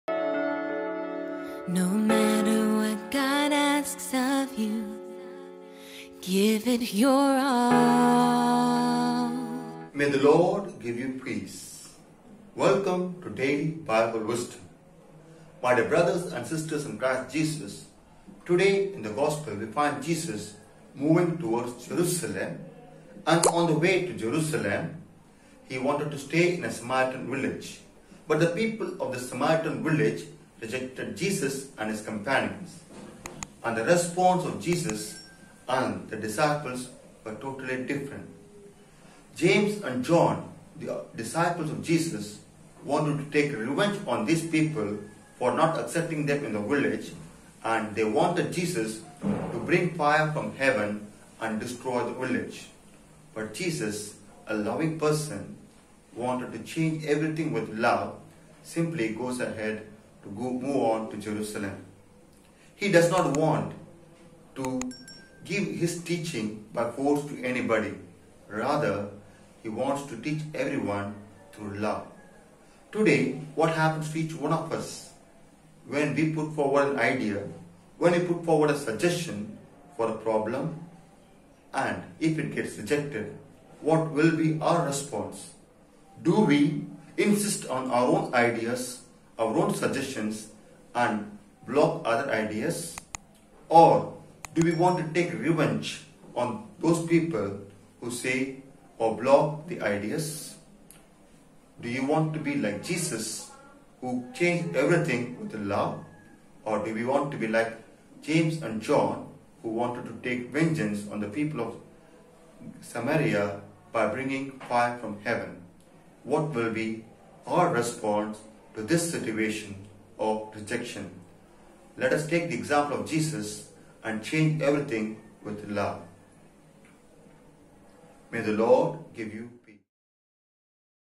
Daily Bible Wisdom | 26 Week Tuesday| JESUS DOES NOT FORCE| Fr Francis A OFM
Friar Francis A ministering in St. Clare's Church in Salmara in the diocese of Bongaigaon in the state of Assam breaks the word for.this week. An enthusiast missionary and pastoral minister...